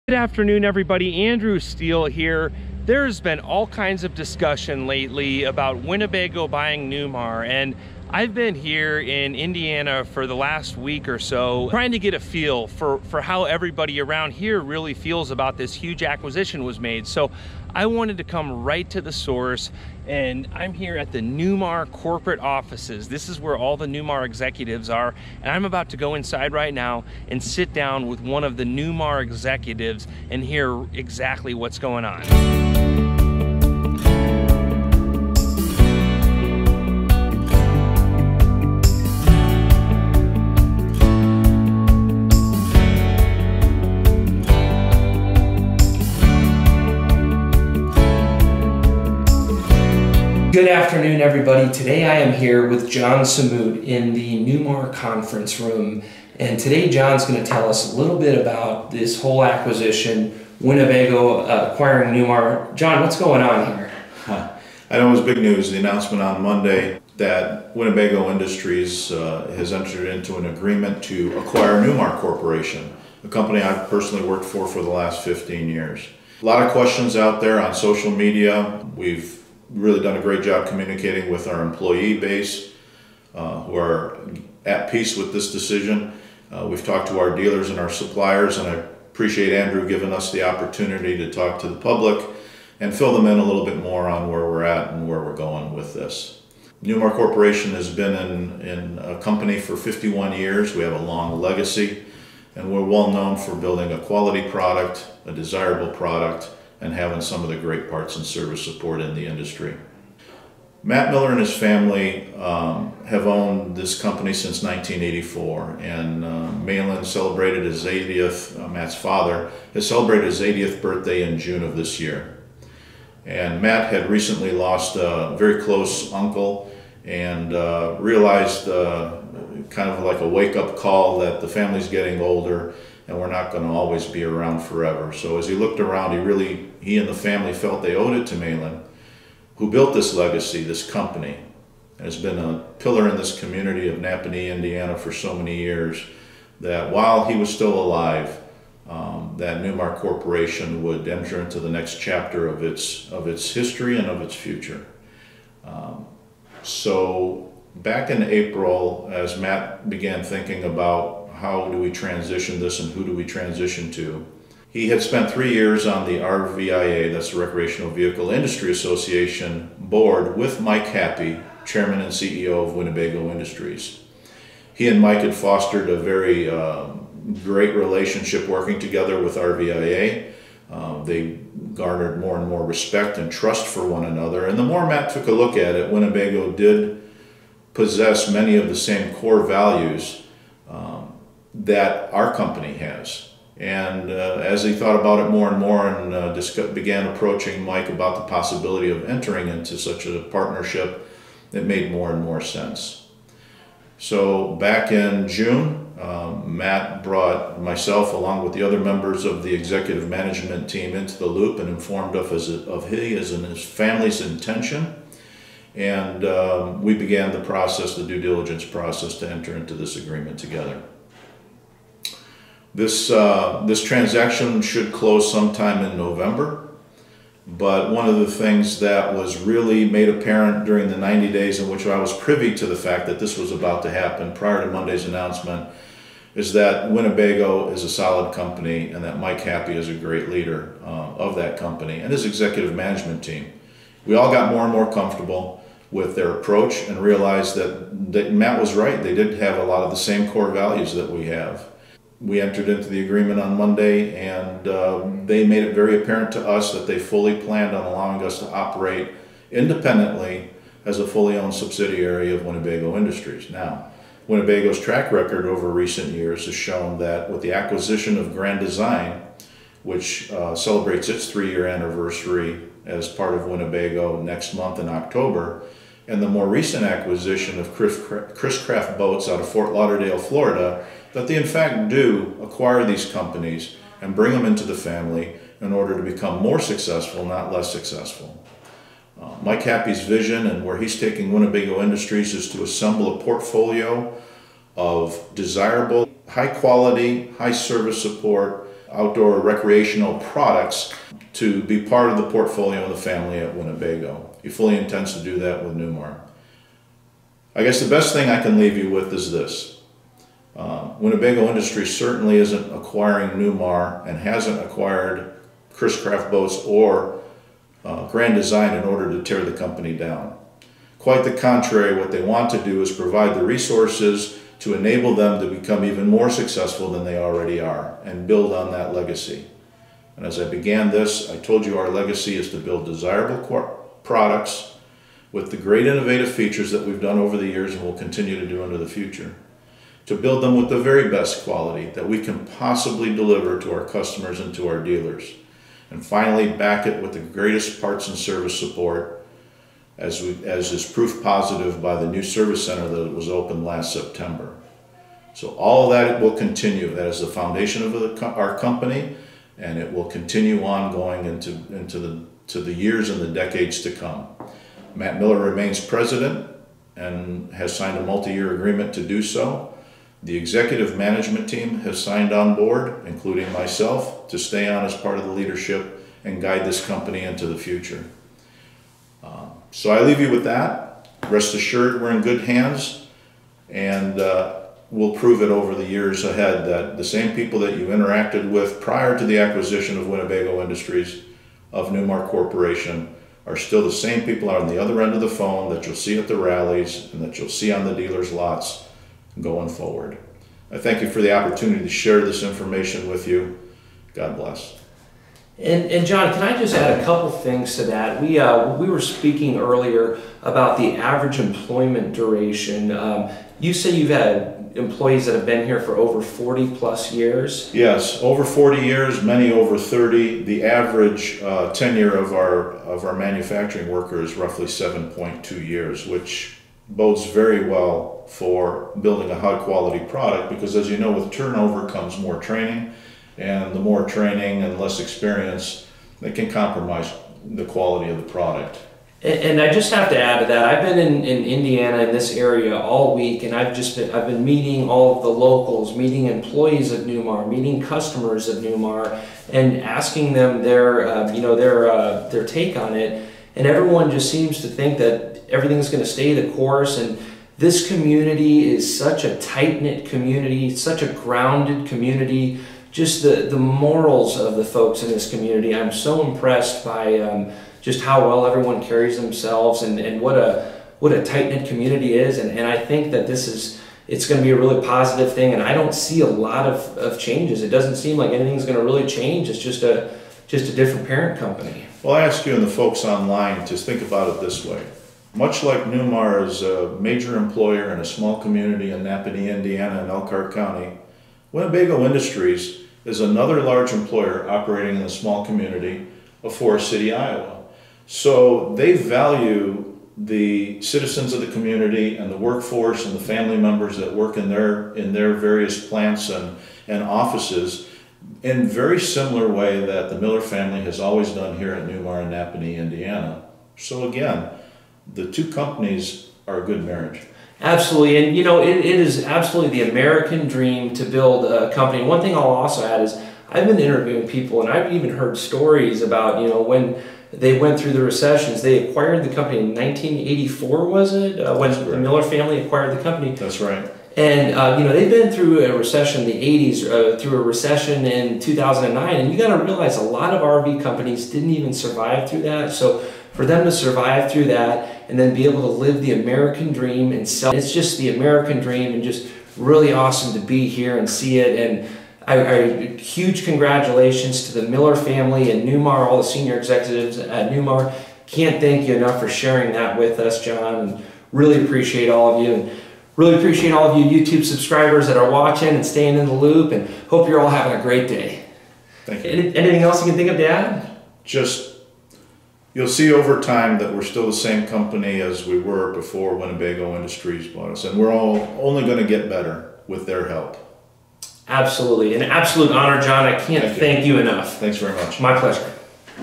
NEWMAR EXECUTIVE TALKS ABOUT WINNEBAGO AQUISITION
In this video, John Sammut, Vice President of Sales and Marketing for Newmar Corp., talks about the recent acquisition of Newmar by Winnebago.